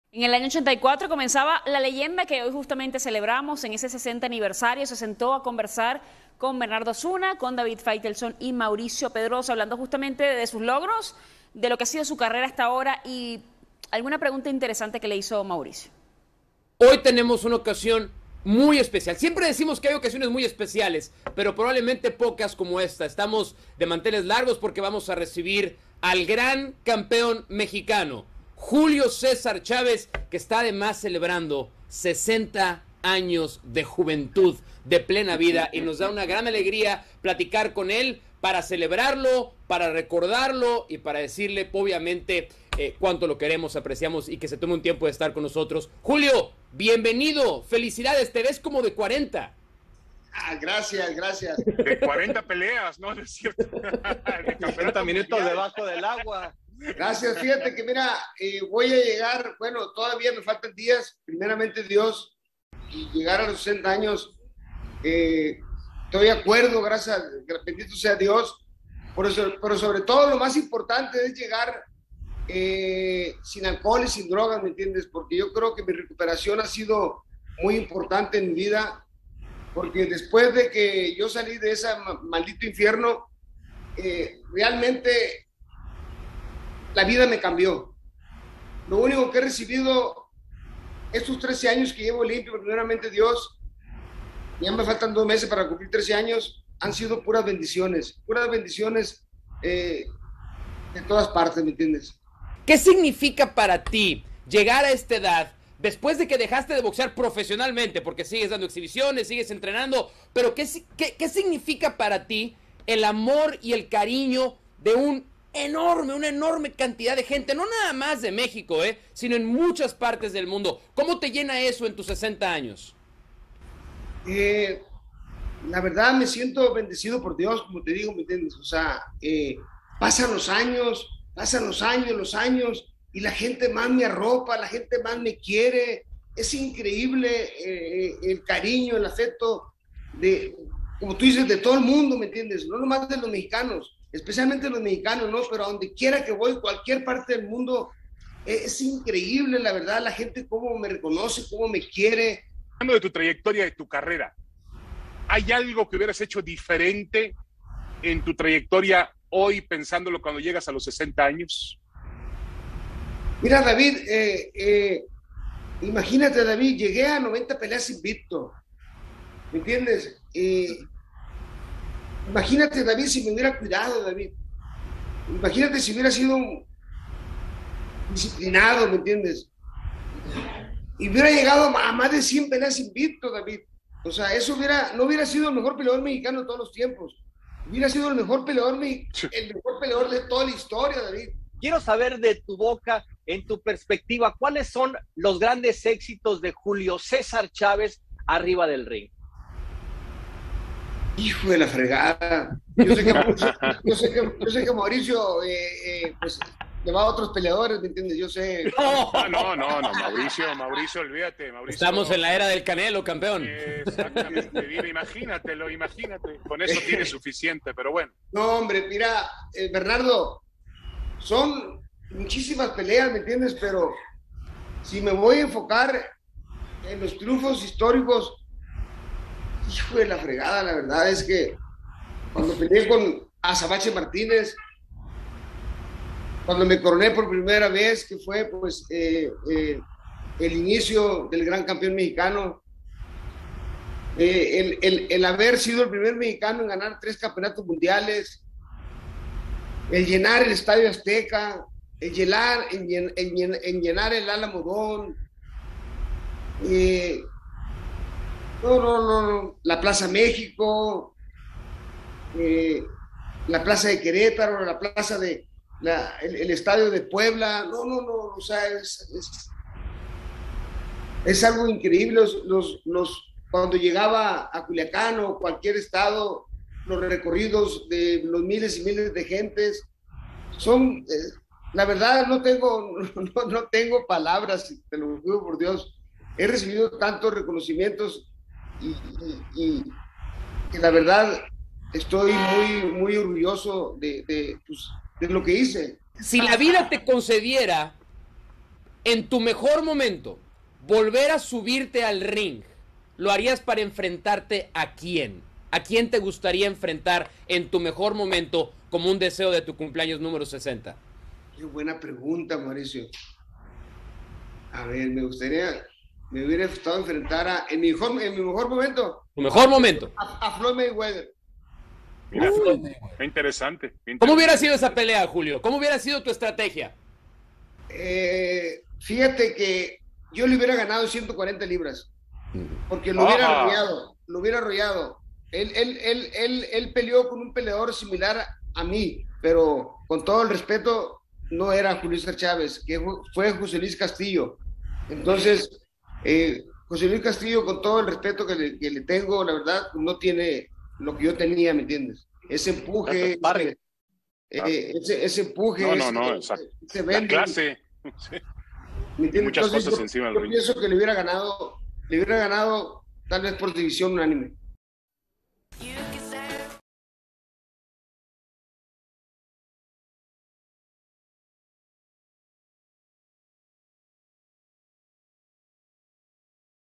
¡Contra Floyd Mayweather! La pelea que le hubiera gustado tener a Julio Cesár
El boxeador mexicano cumplió 60 años y reveló cuál sería el rival que le hubiera gustado enfrentar en su mejor momento.